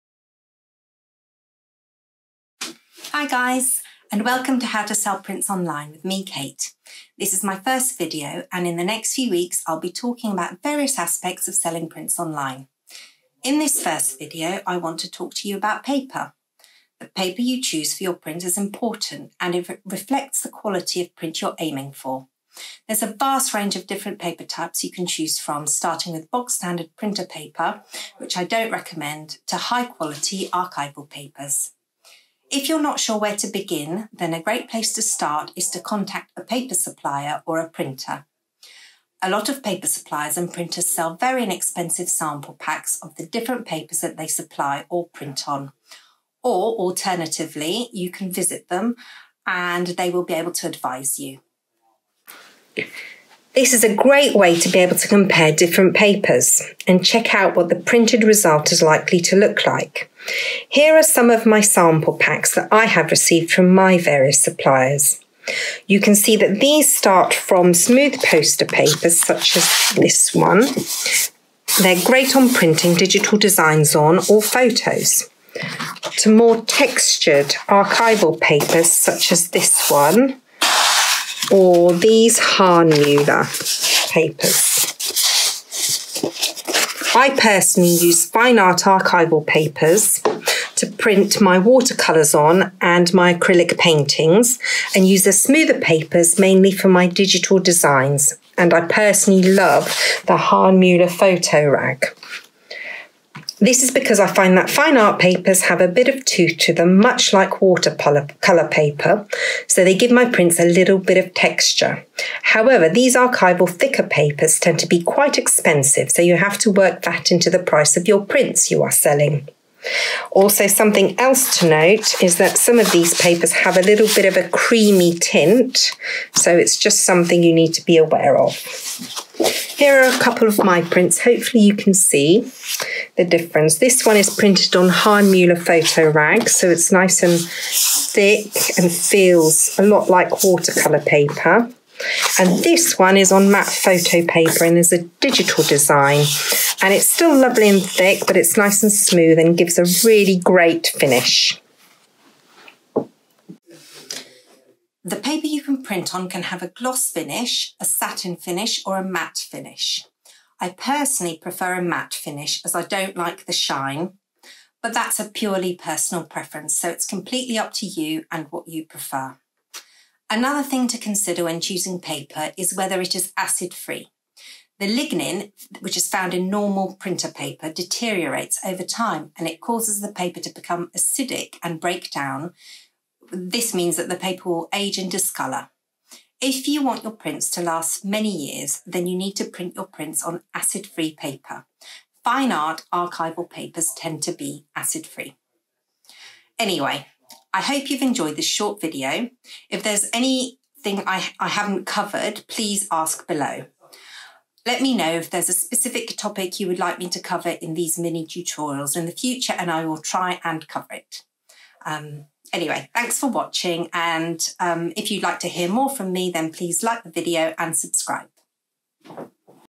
How To Sell Prints Online - All about paper
A video about paper choices when selling prints. This is my first video so please excuse the sound quality - I don't have all the gear!
The paper I mention in this one is the Hahnemuhle Photorage and can be found here :